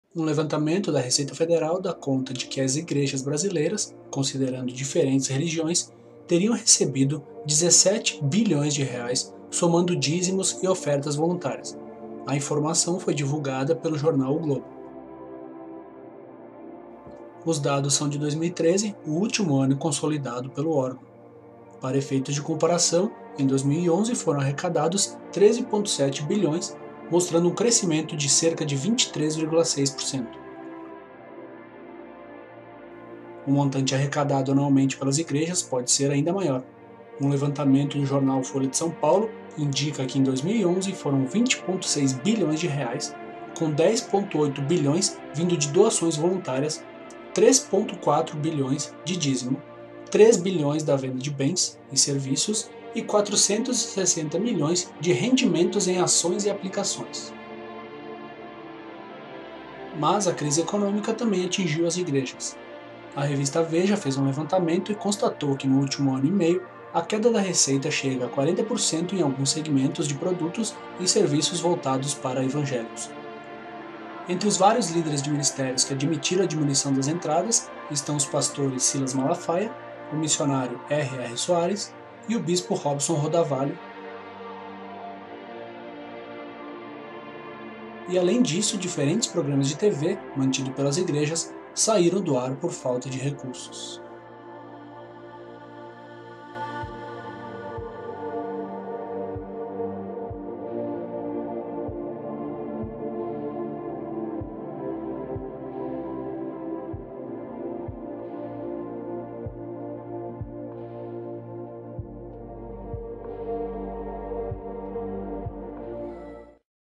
Igrejas arrecadam mais de R$ 17 bilhões por ano no Brasil
Um levantamento da Receita Federal dá conta que as igrejas brasileiras, considerando diferentes religiões, teriam recebido R$ 17 bilhões, somando dízimos e ofertas voluntárias. A informação foi divulgada pelo jornal O Globo.